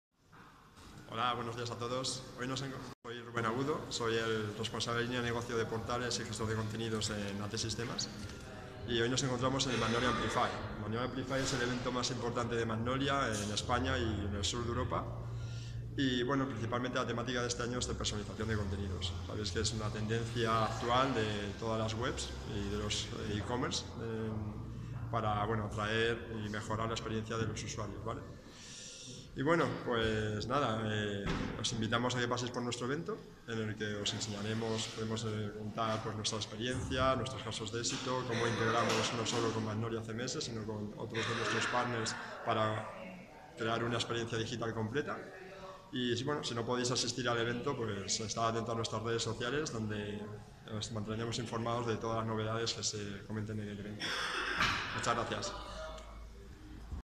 MagnoliaAmplify 2017| atSistemas
Nuestros compañeros Rubén Agudo y Javier Díaz Parrilla, han acudido a MagnoliaAmplify, el evento más importante de Magnolia CMS. Este año, se centra en la personalización de la experiencia de usuario.